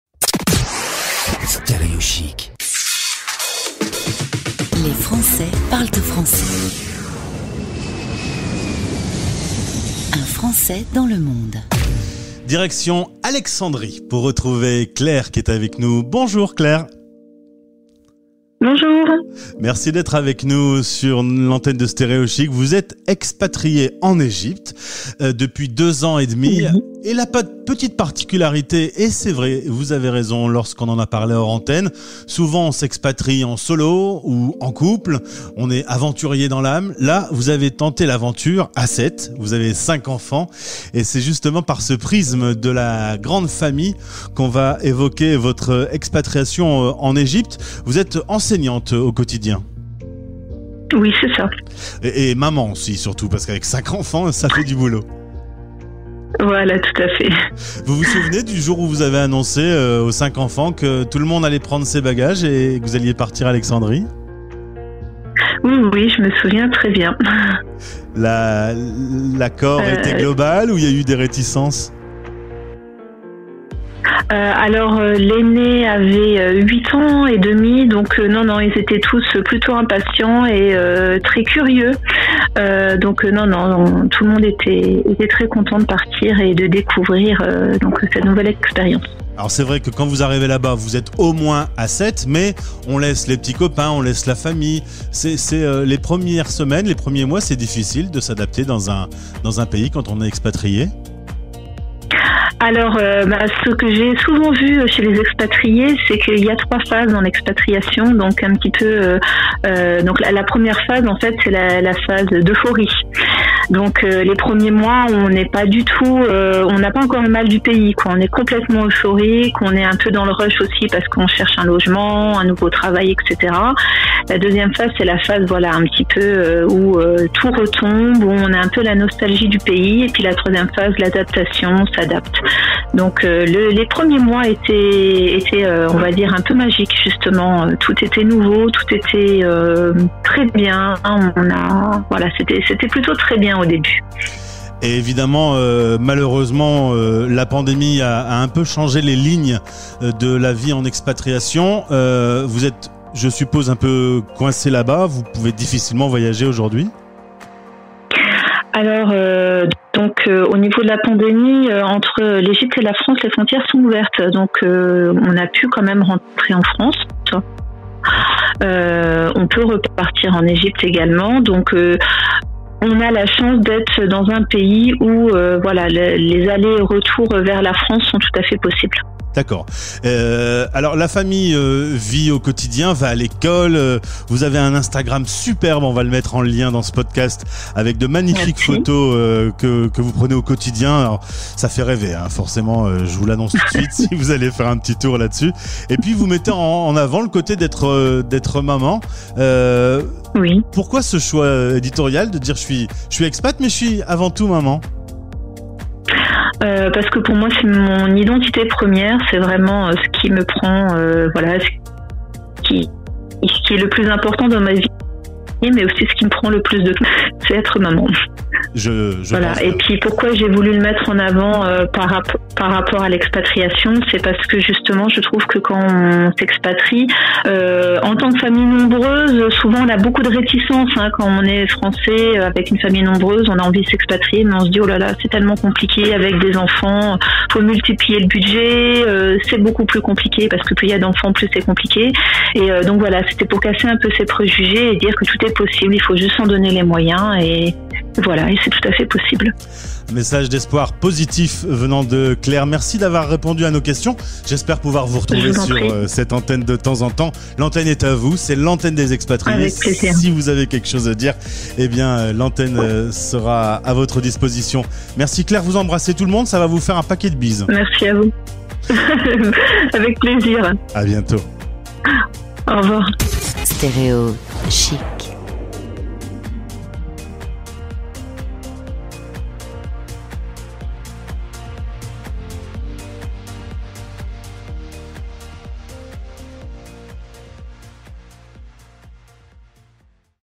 Claire à Alexandrie (Egypte) : une famille de 7 expatriés - 10 11 2020
Cette fois, la chronique "Un Français dans le monde" oublie un peu la vie des expatriés pendant la pandémie pour aborder un autre sujet : Souvent seul ou en couple, quitter la France pour vivre à l'étranger avec une famille nombreuse est un cas un peu plus rare :

Claire, son mari & leurs 5 enfants sont parti de l'Est de la France il y a 2 ans et demi pour s'installer à Alexandrie en Egypte. Enseignante, Claire mène sa carrière et une jolie famille nombreuse dans un quotidien qui a changé : écoutez son récit !

Afrique egypte